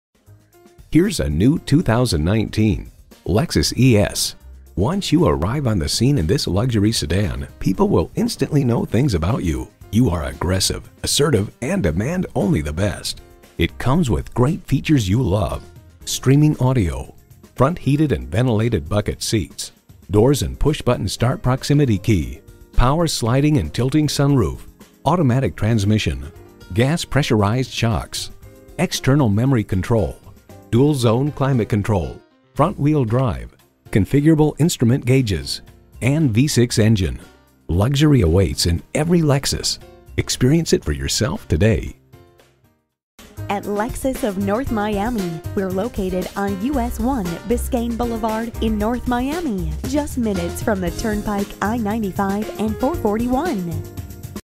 New 2019 Lexus ES Miami, FL #9218700
Year: 2019
Make: Lexus
Model: ES
Trim: 350
Engine: 3.5L V6 DOHC Dual VVT-i 24V
Transmission: 8-Speed Automatic
Color: Gray
Interior: Chateau Nuluxe (Ld)
Stock #: 9218700
VIN: 58ABZ1B13KU016980
Nebula Gray Pearl 2019 Lexus ES 350 FWD 8-Speed Automatic 3.5L V6 DOHC Dual VVT-i 24V Chateau Nuluxe (Ld).brbrRecent Arrival! 22/33 City/Highway MPG

Address:
14100 Biscayne Blvd